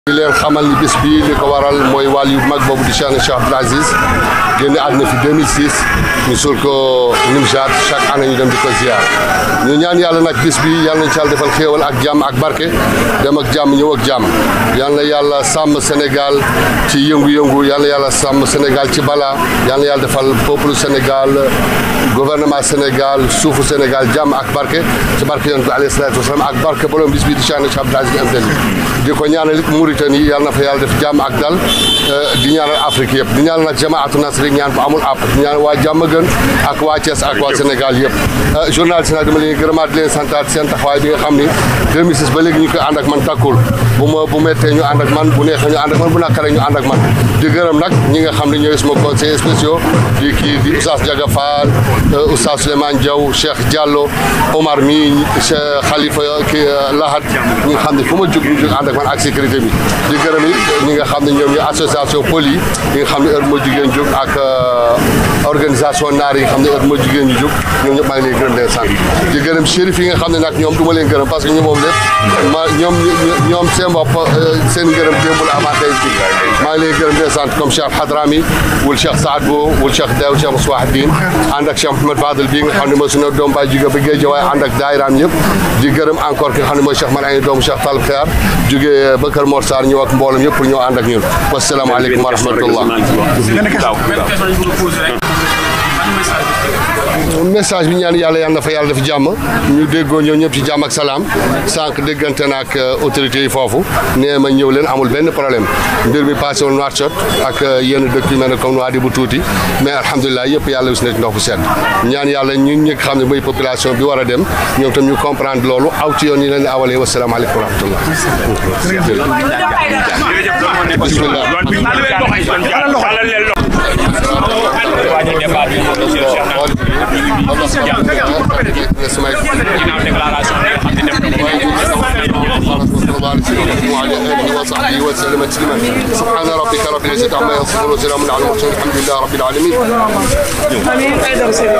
Nimzatt : Le Khalife Général des Khadres appelle à la Paix et exprime son soutien à la Mauritanie
En partance pour la Mauritanie, où sera célébrée la Ziarra annuelle de Nimzatt dédiée à Cheikhna Cheikh Abdoul Aziz Aïdara (petit-fils de Cheikhna Cheikh Saadbou et fils de Cheikh Talibouya), le Khalife général des Khadres, Cheikhna Cheikh Nahma Aïdara, a tenu à délivrer un message de paix et de stabilité. Depuis son quartier à Diamaguène, Thiès, il a exprimé son soutien à la Mauritanie suite aux récentes violences ayant causé plusieurs victimes et entraîné de nombreuses arrestations.

Le guide religieux a également prié pour le Sénégal et pour la Oumma. Sa délégation a été escortée jusqu'à la sortie de Thiès...

Mounirou Mbengue#leraltv